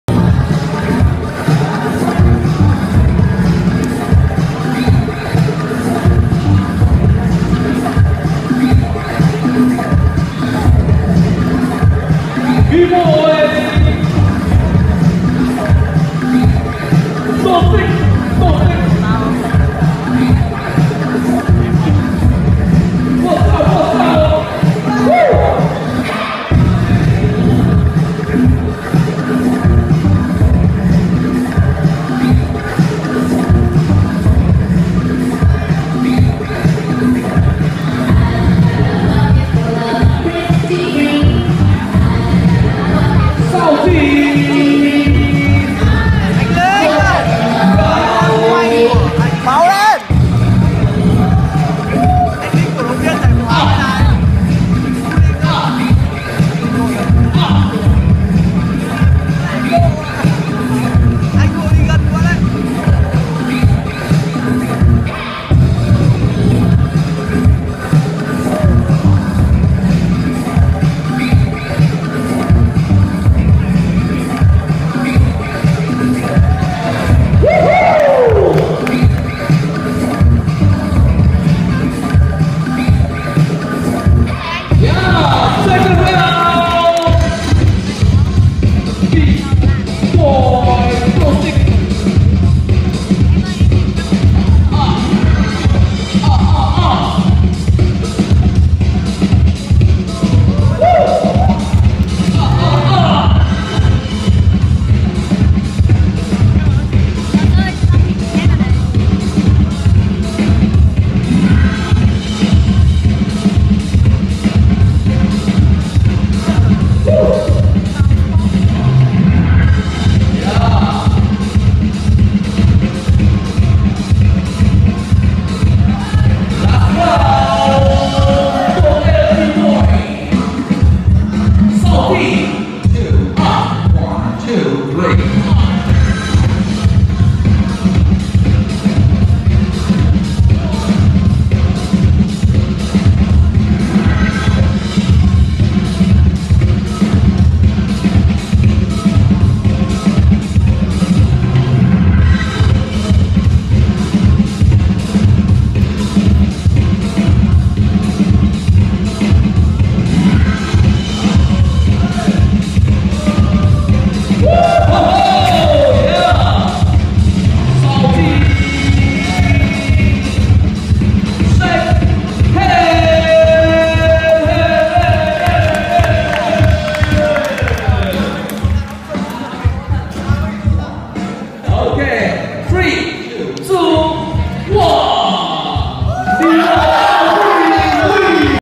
UDU(12-2015)/Bboy1vs1/Top8/Bboy Toxic vs Bboy South Tee
Winner: Bboy South Tee
Undergrown Up competition
SUNDAY 6th Dec 2015
Showcase team & Bboy battle 1vs1
Kid battle - House / Freestyle
Judge:
- CK ANIMATION / Flow
- AKILLA / Big Toe
- RÙA / New York Style
- MAITINHVI / CunCun (Big Toe)
- TUNE TUN / Last Fire (S.i.n.e)
- SLOWZ - (2Groove/Natural Groove)
- SPIN - S.i.n.e
Champion
- Showcase: Wonder Woman
- BBoy battle: TTN
- Kid House: Dai Nghia
- Kid freestyle: Bich Ngoc